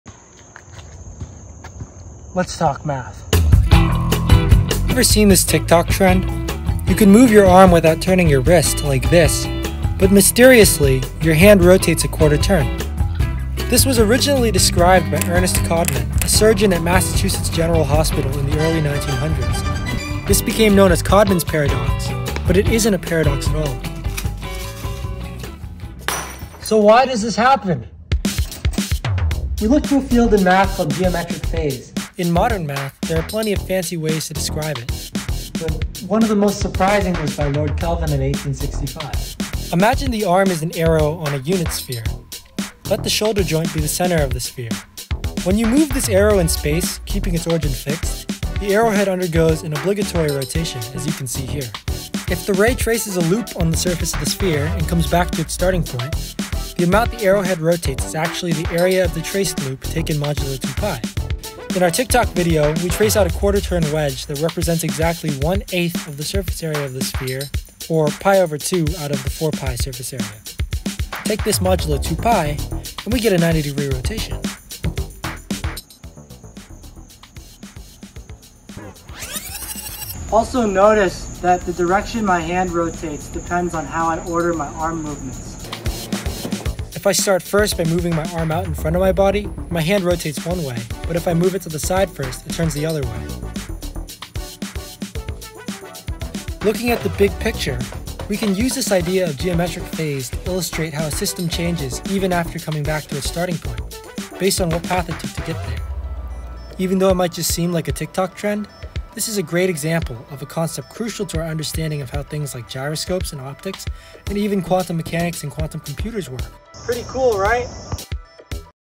What is Geometric Phase? | Breakthrough Junior Challenge 2025
Have you seen these TikTok videos? Codman's Paradox is an implicit rotation that occurs in the shoulder joint as the arm moves, and is an example of what is known in math as Geometric Phase.